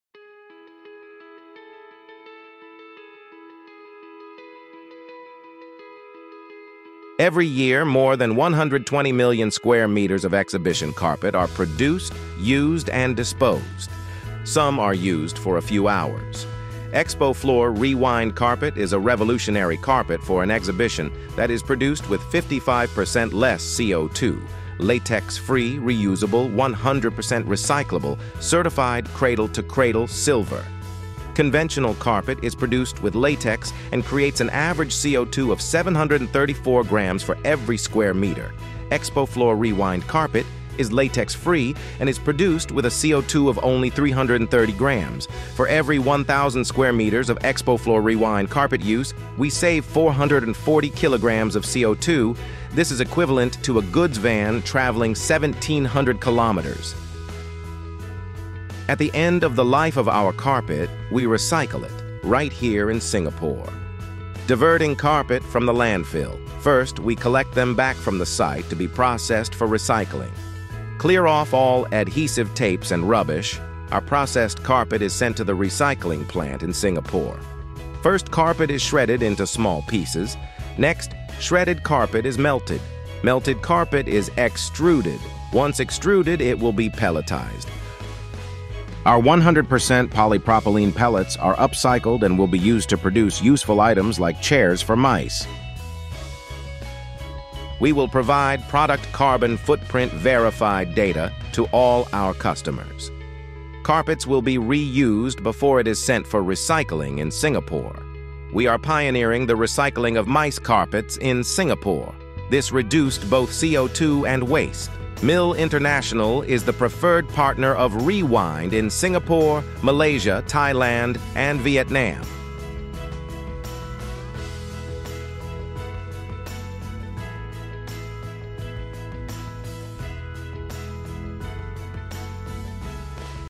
MICE Carpet Recycling Singapore
Every year, more than 120 million square meters of exhibition carpet are produced, used, and disposed. Watch on to learn more about Expoflor REWIND carpet and see how we recycle our carpets in Singapore.